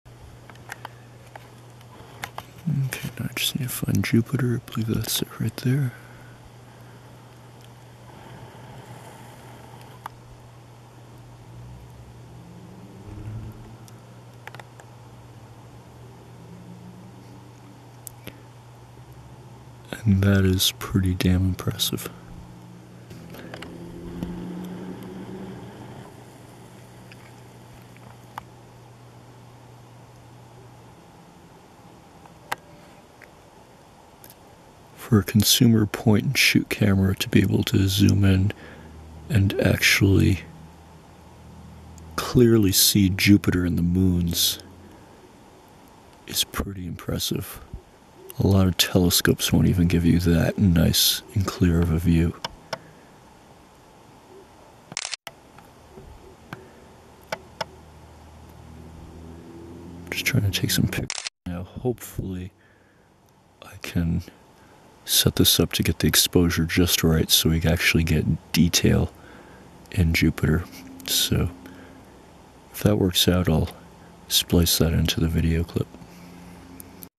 Canon Powershot SX50 HS Jupiter & Moons 200x Zoom Test Hand Held
Well Finally after a month and a half, Jupiter is out, the sky is clear, and I was able to get a nice zoom test video. I wasn't expecting a whole lot, to so clearly be able to see Jupiter and its moons was quite impressive. Its also a good demonstration of how stable the video is even under high zoom, while not on a tripod.

This is not an astro-photography camera however, it does do a wide range of things but you wont be able to get pics of galaxies and things like that until canon does a firmware upgrade removing some manufacturer implemented ISO / Exposure limitations.